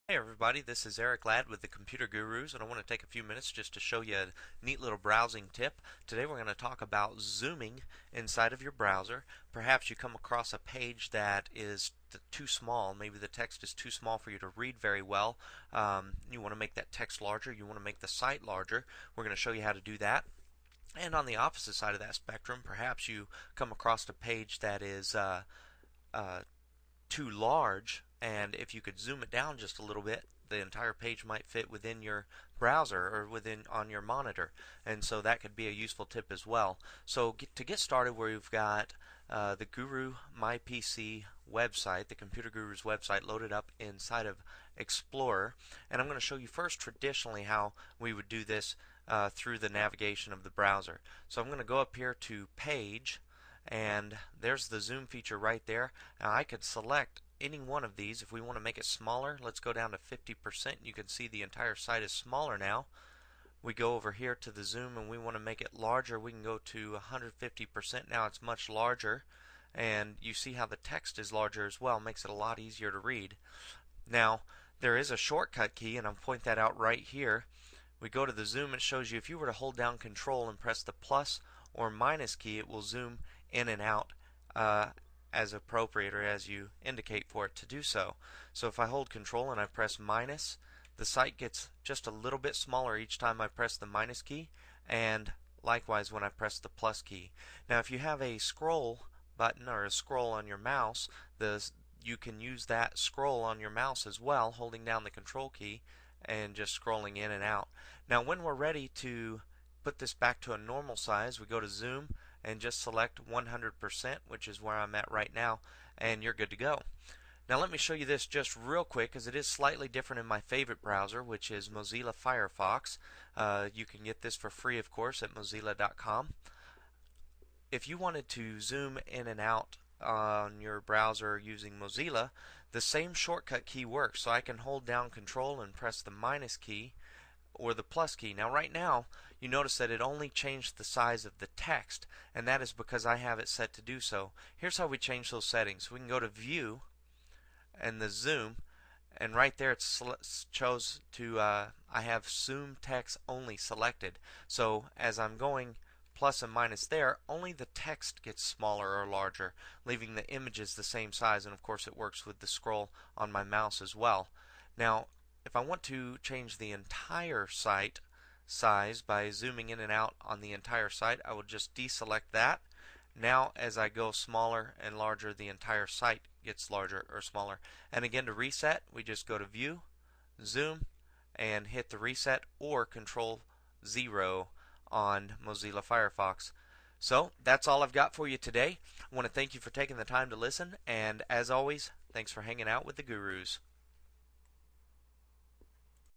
Guru Tips - Internet Browsing - Zoom
We cover basic use of the zoom feature while browsing the internet in both Internet Explorer and Mozilla Firefox. Make a site appear larger or smaller to better accommodate your viewing preference. For example if text on a site is too small to read you can make it larger by using the browser zoom feature. We also cover how to reset browser zoom for proper viewing of other sites after viewing the one you have adjusted.